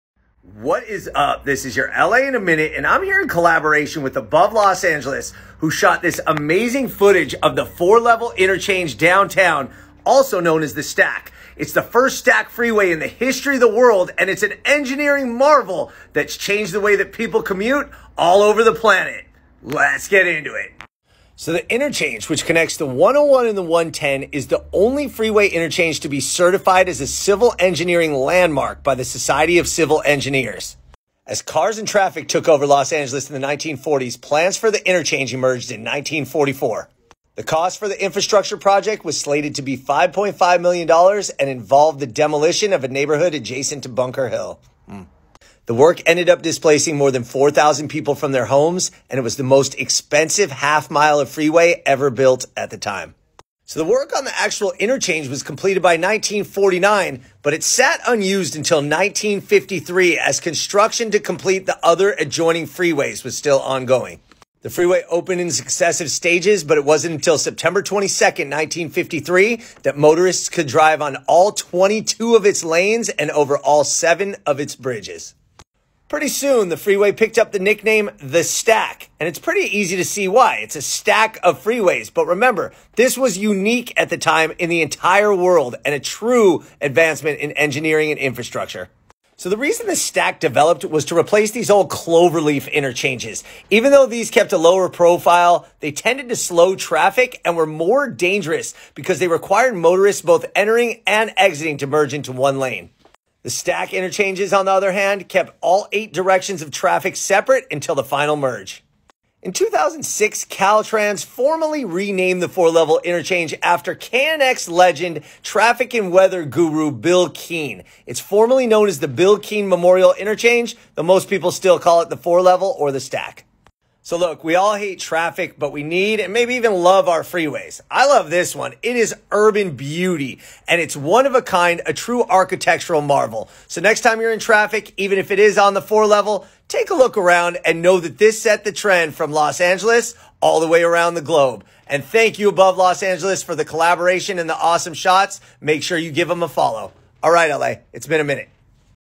The Four-Level Exchange: the Freeway that Changed the World!
The Four-level Interchange in downtown Los Angeles is an engineering & architectural marvel that has changed commuting in Los Angeles and the world. It is the first stacked freeway and is a major artery in Los Angeles driven by hundreds of thousands of computers per day. Let’s get into it! Thank you Above Los Angeles for the awesome footage and collab.